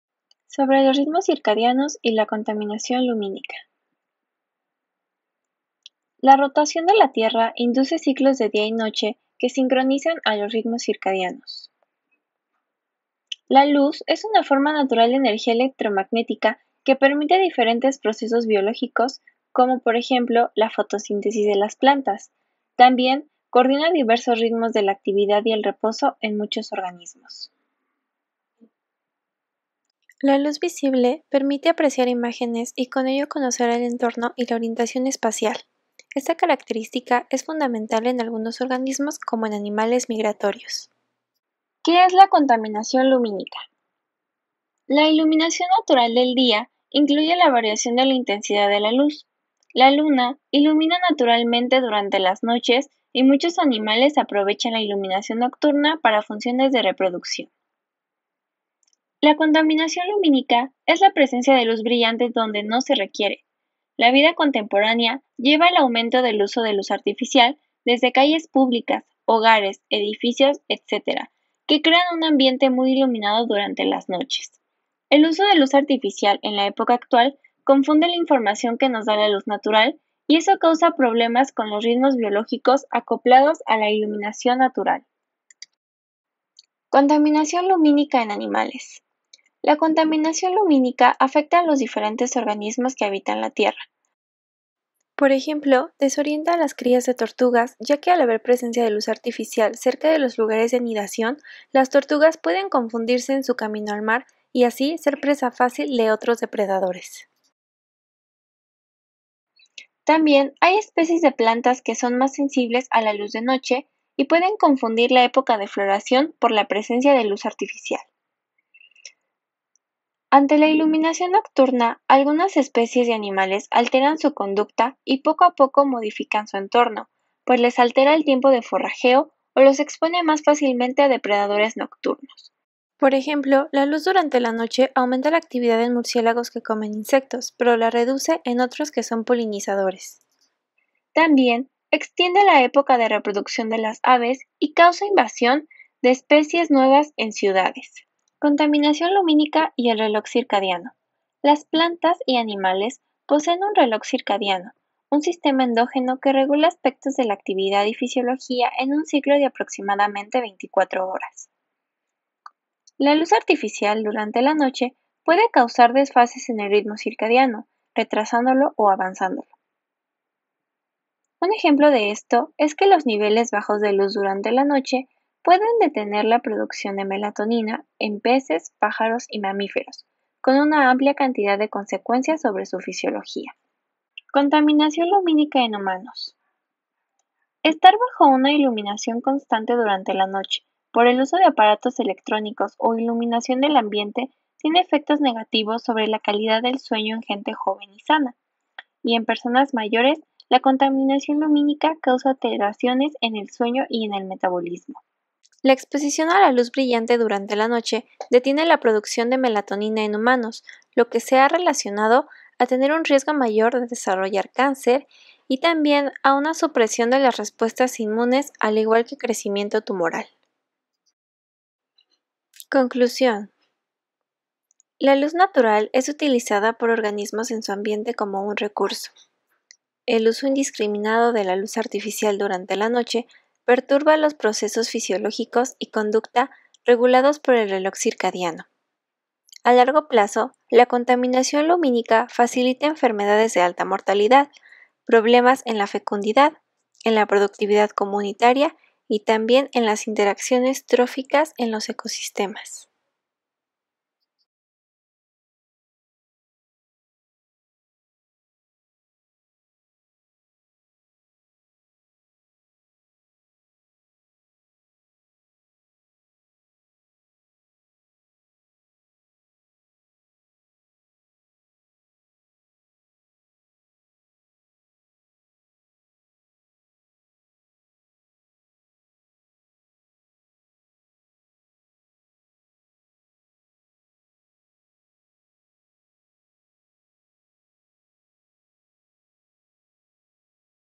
Sobre los Ritmos Circadianos y la Contaminación Lumínica
Video del Laboratorio de Cronobiología de la UMDI-Juriquilla Facultad de Ciencias UNAM.